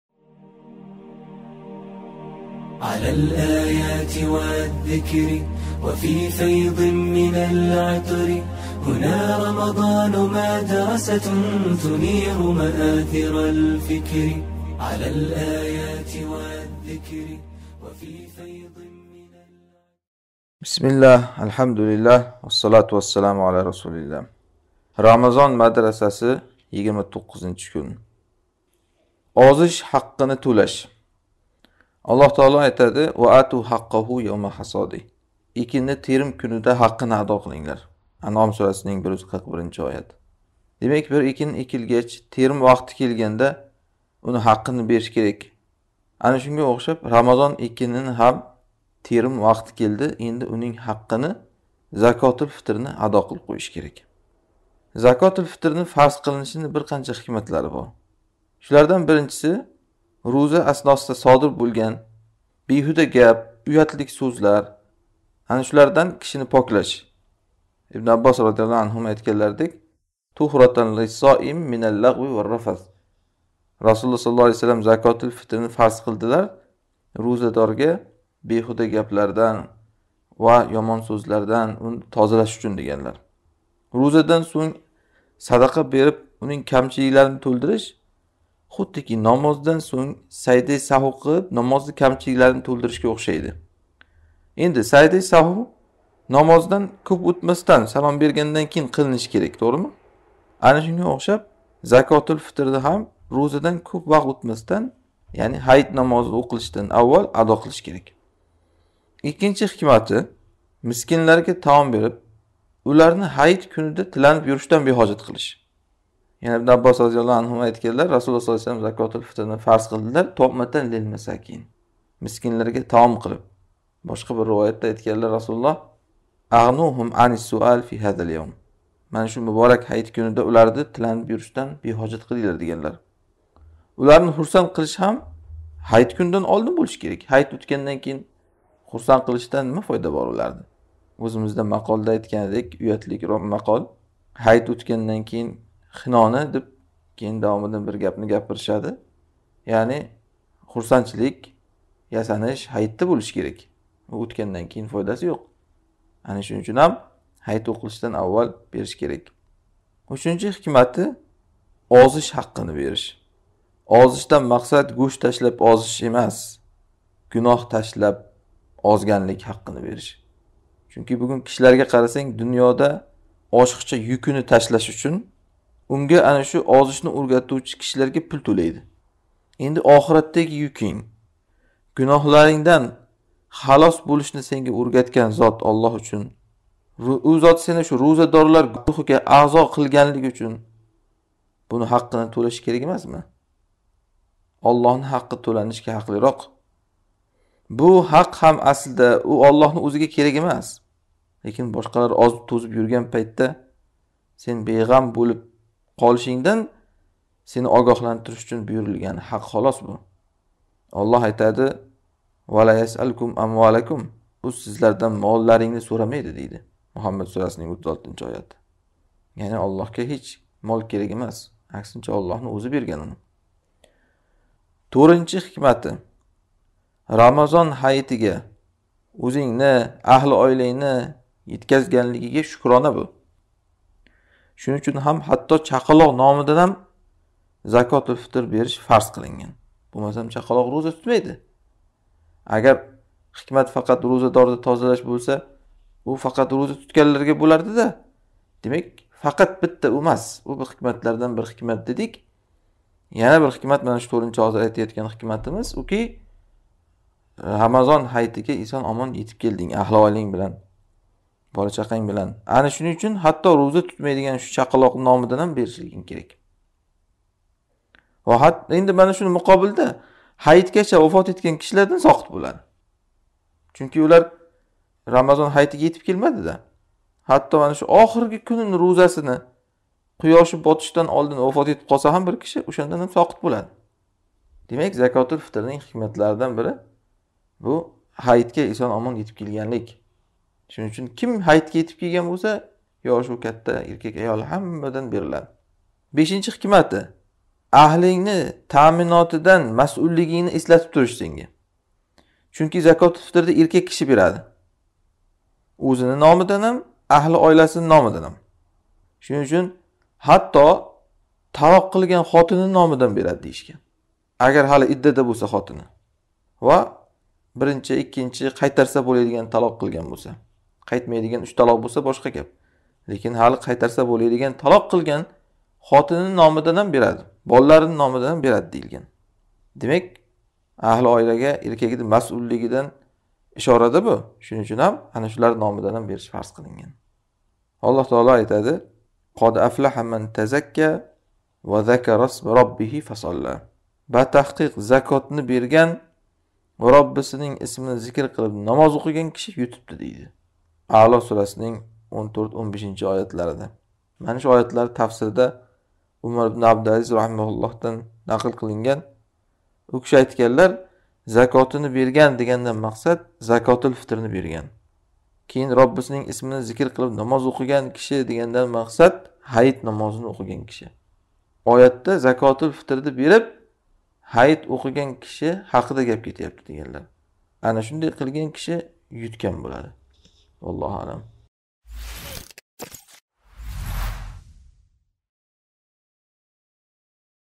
Ramazon madrasasi 29-dars ll Ozish haqqini to‘lash ll Ustoz Abdulloh Zufar
Ayollar uchun maxsus darsliklar